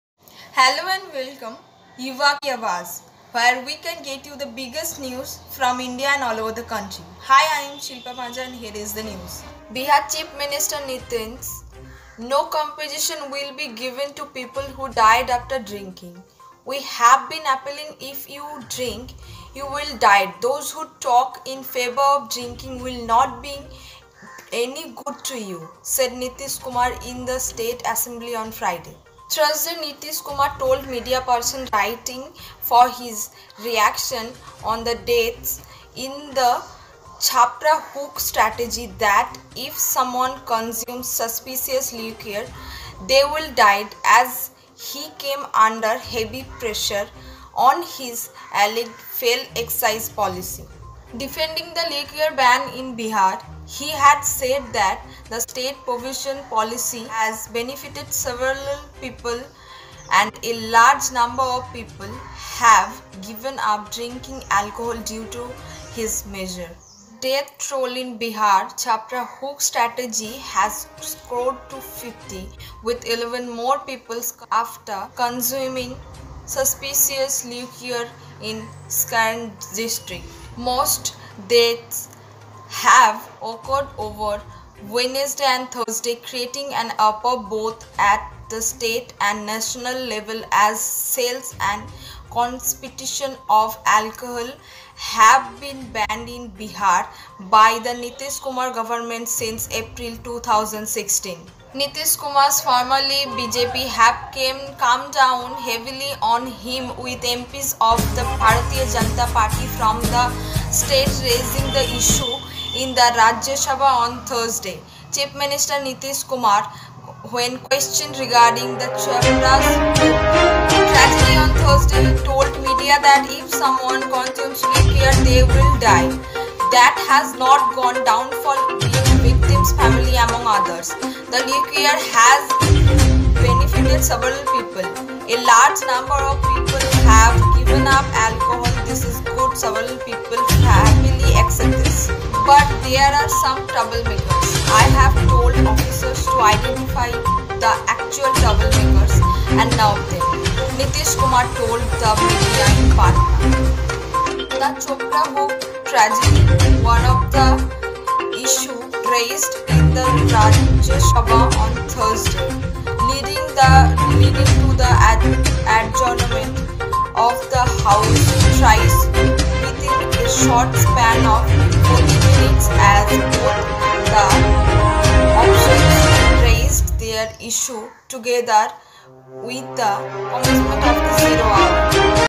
Bihar alchohal Ban : No Compensation for people who drink alcohol and die Nitish Kumar | VOY Nation
CM of Bihar said - If you Drink you will die
Bihar Chief Minister Nitish " No compensation will be given to people who died
after drinking ... We have been appealing- if you drink , you will die those who
talk in favour of drinking will not bring any good to you " , said Nitish Kumar in
the state assembly on Friday.Thursday , Nitish Kumar told media persons
waiting for his reaction on the deaths in the Chhapra hooch tragedy that if "
someone consumes spurious liquor, they will die"as he came under heavy
pressure on his alleged failed excise policy.Defending the liquor ban in Bihar ,
he had said that the state's prohibition policy has benefitted several people
and a large number of people have given up drinking alcohol due to his
measures .The death toll in Bihar's Chhapra Hooch tragedy has soared to 50 ,
with 11 more people succumbing after consuming spurious liquor in Saran
district .
Most deaths have occurred over Wednesday and Thursday creating an uproar
both at the state and national level as sale and consumption of alcohol have
been banned in Bihar by the Nitish Kumar government since April 2016. Nitish
Kumar's former ally , BJP , have come down heavily on him with MPs of the
Bharatiya Janata Party from the state raising the issue in the Rajya Sabha on
Thursday . Chief Minister Nitish Kumar , when questioned regarding the
Chhapra Hooch .Tragedy , on Thursday told media that " if someone consumes
liquor , they will die ", that has not gone down well with victim families among
others . The liquor ban has benefitted several people . A large number of
people have given up alcohol, This is good . Several people have happily
accepted this . But there are some troublemakers . I have told officers to
identify the actual troublemakers and nab them , "Kumar told the media in
Patna . The Chhapra Hooch tragedy was one of the issues raised in the Rajya
Sabha on Thursday , leading to the adjournment of the House thrice within a
short span of 40 minutes as both the Treasury benches and the opposition
raised their issues together with the commencement of the Zero Hour.